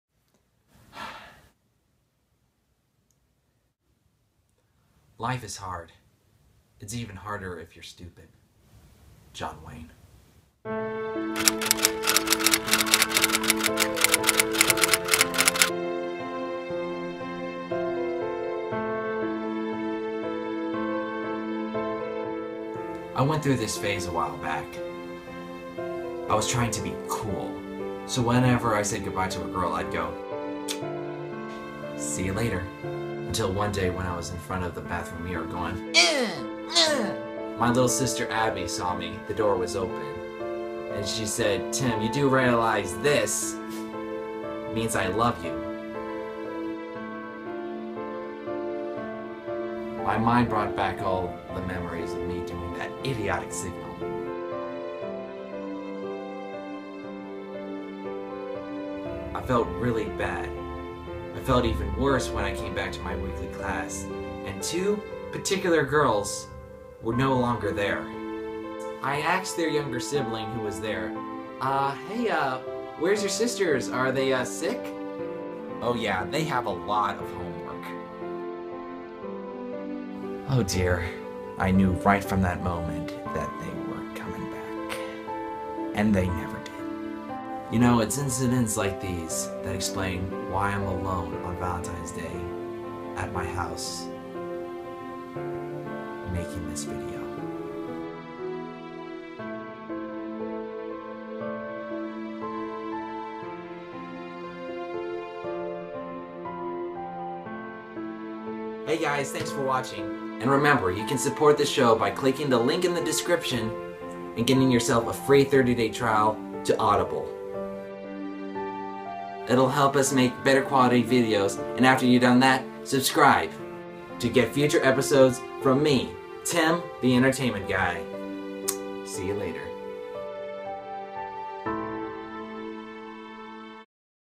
Tim's True Stories: The Awkward Symbol
Tim tells his dramatic story in a black and white setting.